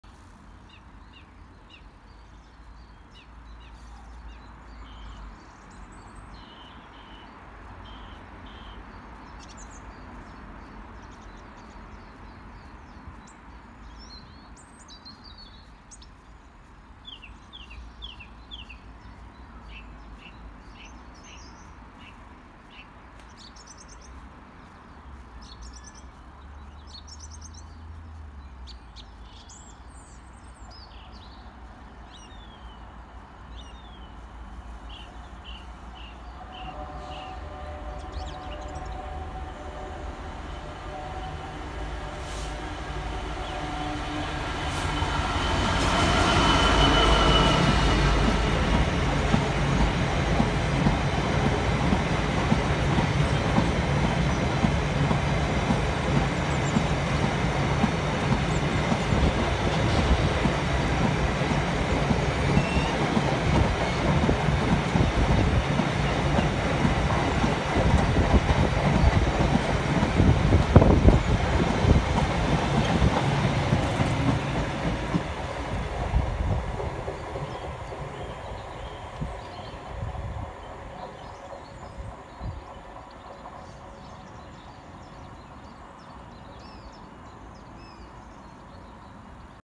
66170 4E25 Bow Depot to Heck Plasmor P S 16:55 12E 19/3/2024
66170 4E25 Bow Depot to Heck Plasmor P S 16:55 running 12 minutes early Lea Road Gainsborough 19/3/2024